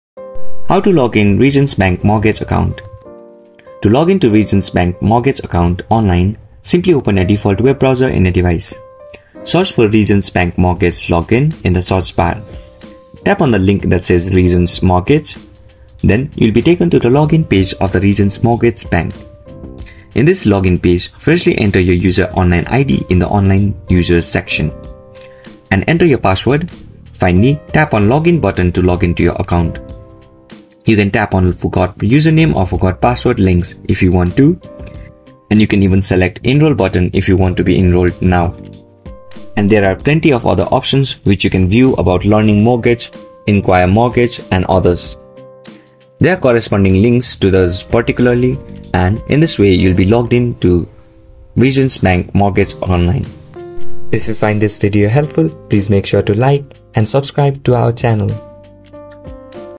How To Login Regions Bank Mortgage Account 2024
Need to access your Regions Bank mortgage account online in 2024? This guide provides a clear, step-by-step process on how to log in to your Regions Bank mortgage account. It’s designed for customers who want to manage their mortgage details conveniently online, from checking balances to making payments.

In this tutorial:
0:00 - Introduction
0:05 - How To Login to Your Regions Bank Mortgage Account 2024 (EASY)

Learn the straightforward method to log in to your Regions Bank mortgage account in 2024 with this comprehensive tutorial. The video guides you through navigating to the Regions Bank website, finding the mortgage login section, entering your account credentials, and accessing your mortgage information securely and efficiently.

Manage your mortgage smoothly by following this guide to log in to your Regions Bank mortgage account in 2024!